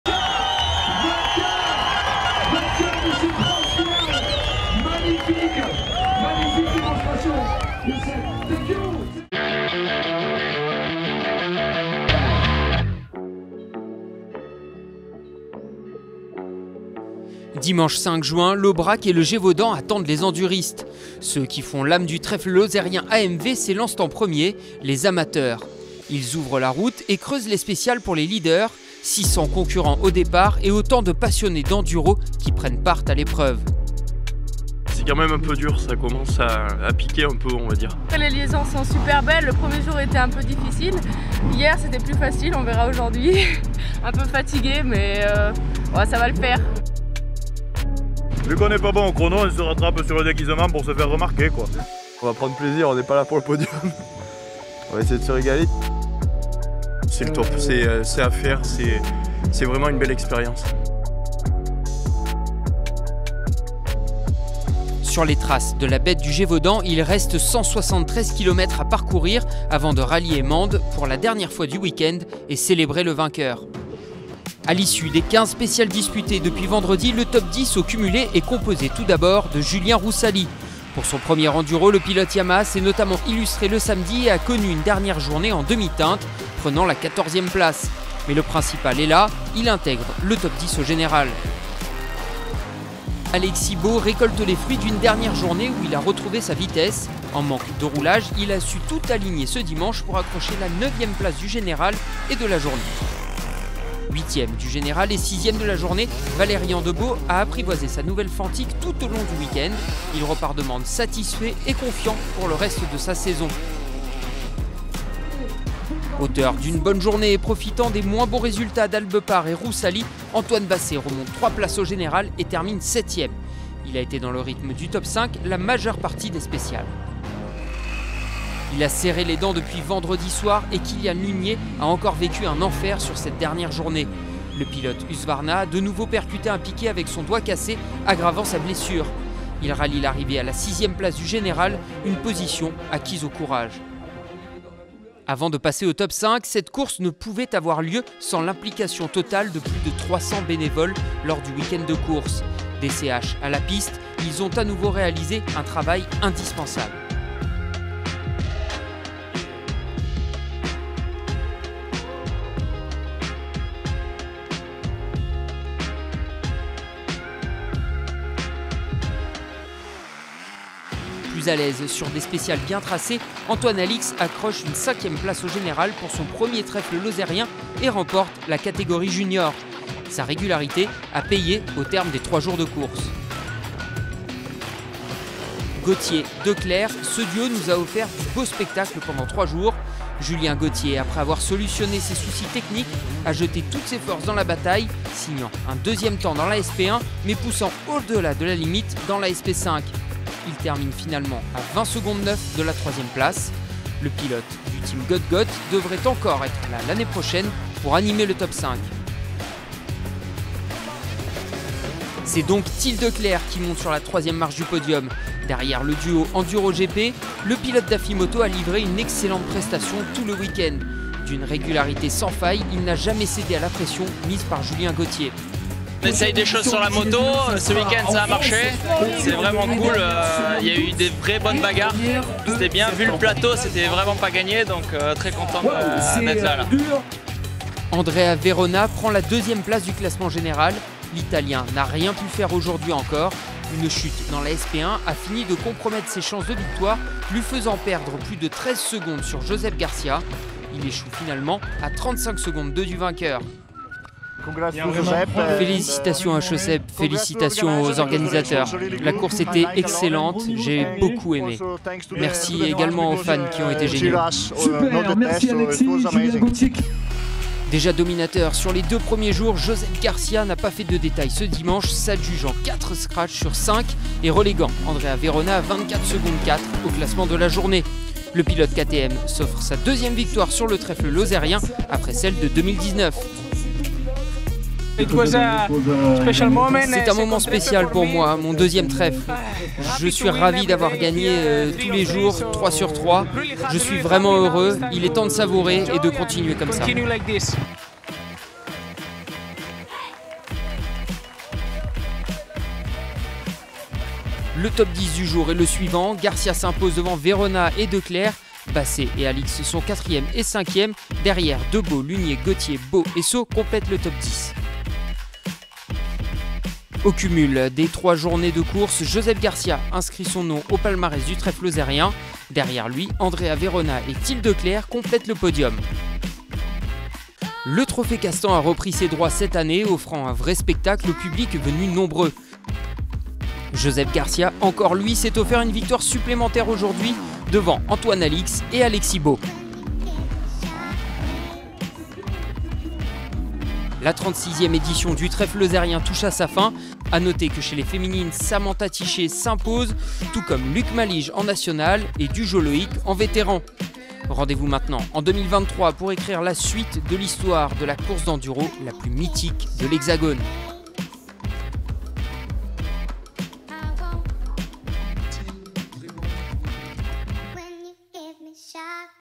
Trefle Lozérien AMV 2022 / Josep Garcia signe sa deuxième victoire !
C’est au terme de la troisième et dernière journée de cette 36è édition du Trèfle Lozérien AMV que Josep Garcia inscrit pour la seconde fois son nom au palmarès de l’épreuve après son succès de 2019. On savait que le départ dans l’ordre inverse des numéros pouvait marquer une réelle différence au niveau des temps et traduction directe, Garcia gagne ce dimanche avec 28 secondes d’avance sur Andrea Verona et conforte son leadership au général.